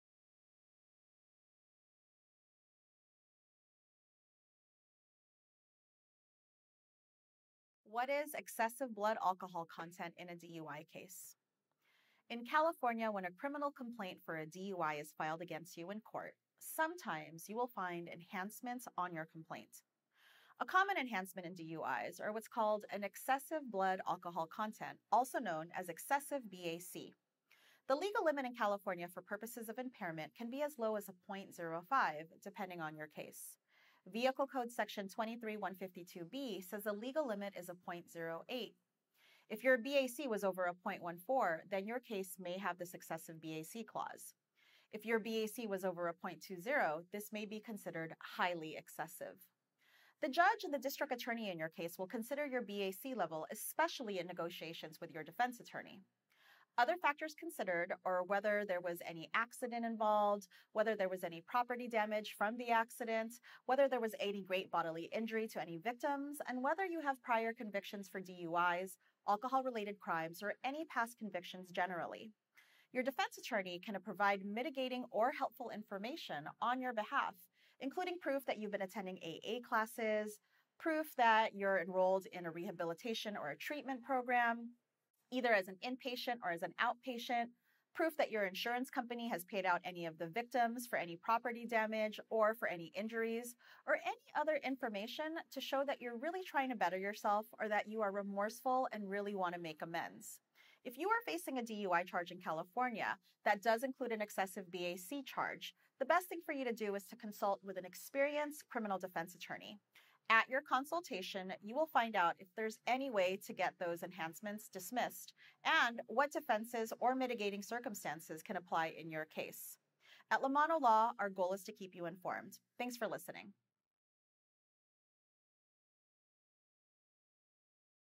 What is Excessive Blood Alcohol Content in a DUI case in California?
When a criminal complaint for a DUI is filed against you in court, sometimes you will find enhancements on your complaints. A common enhancement in DUIs is what's called excessive blood alcohol content, also known as excessive BAC.

What to listen for in this video:
0:00 - Disclaimer
0:05 - What is excessive blood alcohol content in a DUI case?
0:50 - Factors considered in negotiations with the judge & district attorney
1:40 - Best course of action